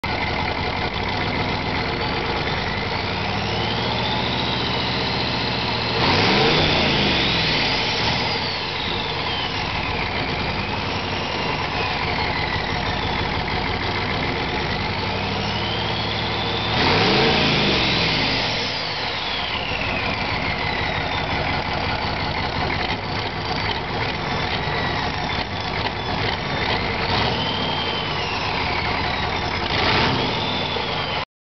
6.2L turbo diesel running
My 1988 6.2L diesel in an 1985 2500 gmc, starting for the second time in about 6 or more months. Since i took the engine out of my other truck, i replaced practically all the seals, the timing chain, and also gave it a paint job.